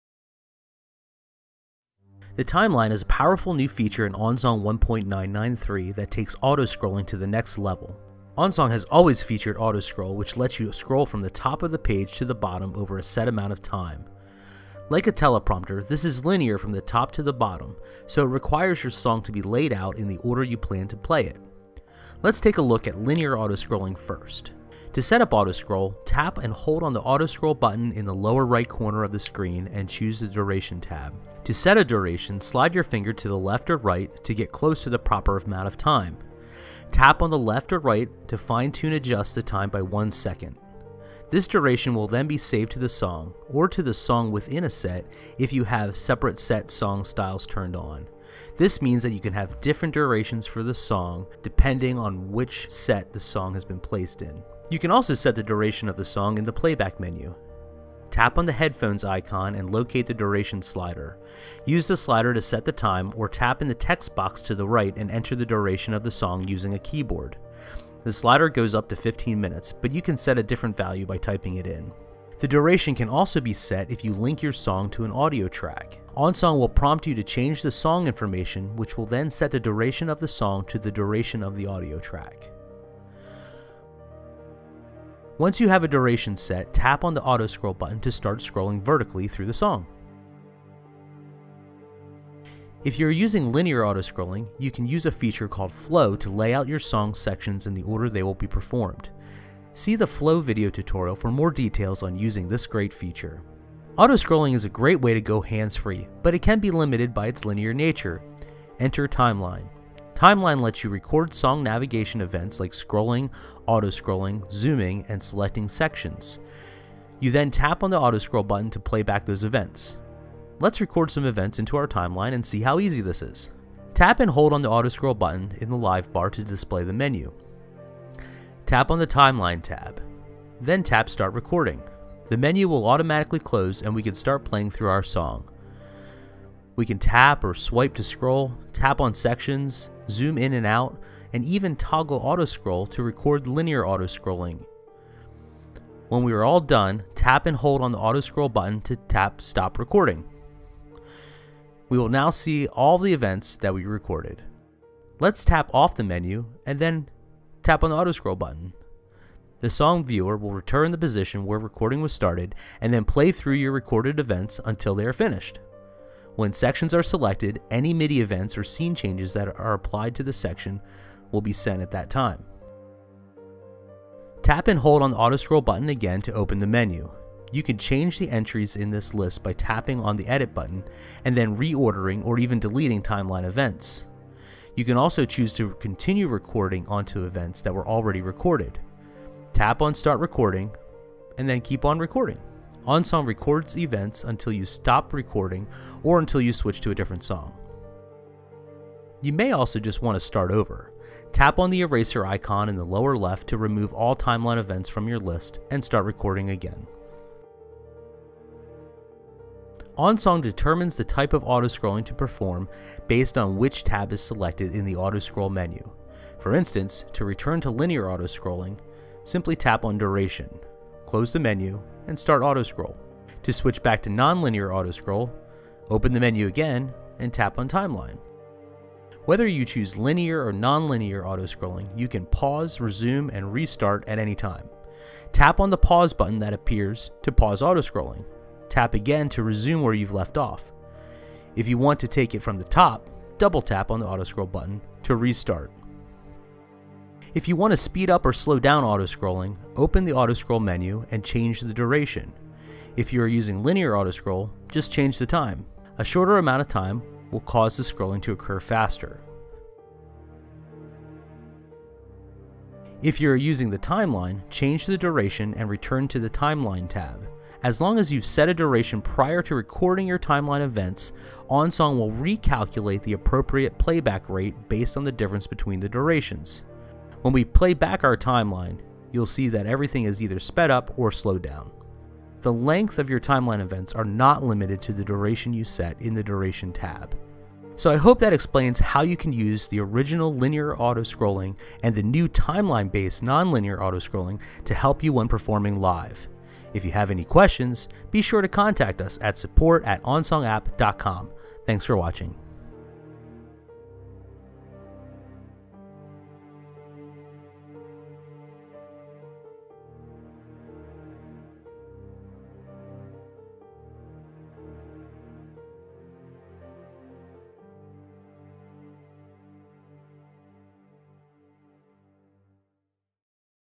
Non-Linear Autoscrolling with Timeline
In this video tutorial we review autoscrolling.  Learn how to set up linear autoscrolling to move from the top of your song to the bottom over a given amount of time.

Jason also reveals a new feature in OnSong 1.993 called Timeline that lets you record navigation events in a non-linear fashion.